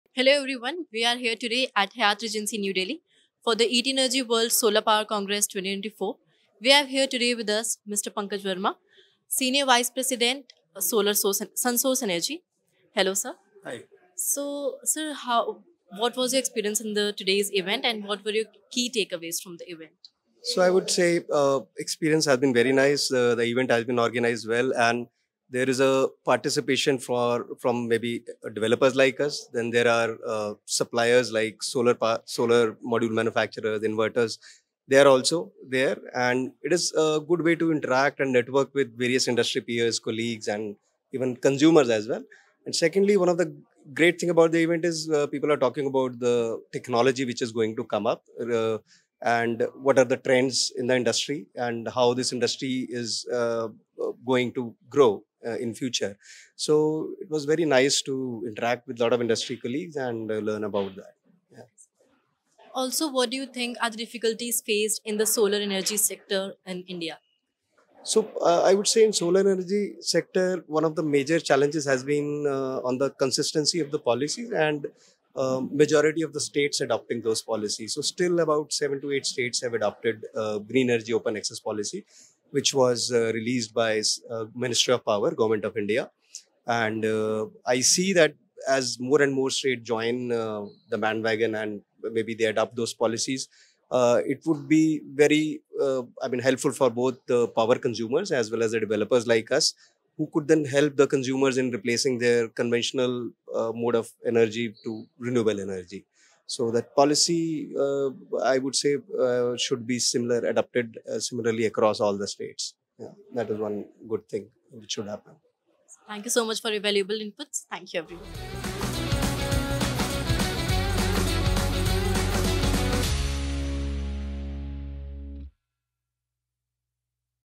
Pankaj Verma at ETSolarCongress!
Discover the insights and reflections shared by our esteemed speaker, Pankaj Verma, Senior Vice President, SunSource Energy on their remarkable journey at ETSolarCongress! 🌟🎙